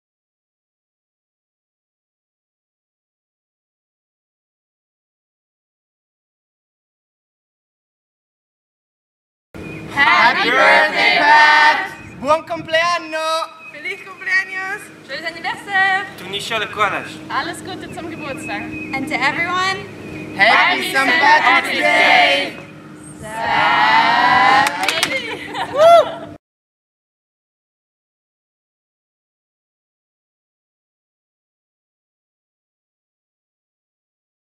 Pat Parelli's Special Birthday Message
A Surprise Birthday Video For Pat made by his Team at The Parelli Talent Development Center